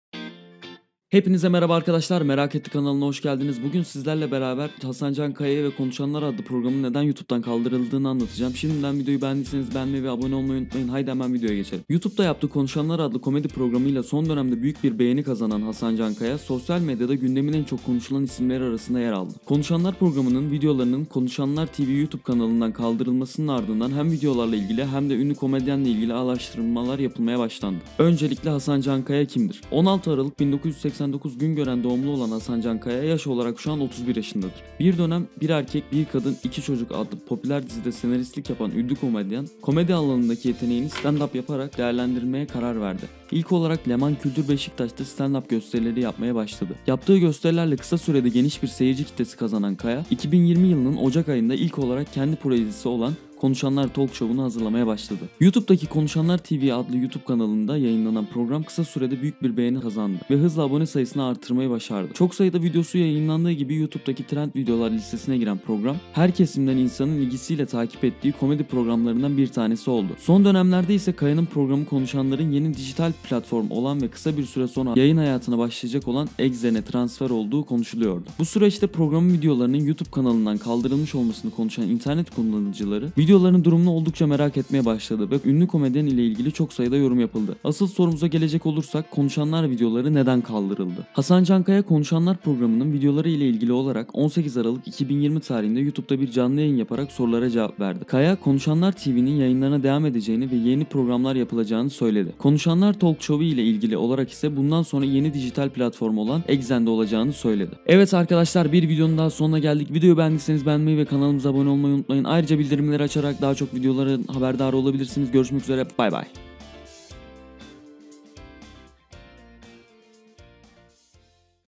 Konuşanlar Neden Kaldırıldı? Hasan Can Kaya Kimdir?
Bu video da Konuşanlar Neden Kaldırıldı? , Hasan Can Kaya Kimdir? , Exxen'e geçiş Yaptı mı? Exxen Platformu, Hasan Can Kaya Exxen' le anlaştı mı? Konuşanlar Nerede Yayınlanıyor? Konuşanlar Programı Kaldırıldı mı? , Cevaplarını verdik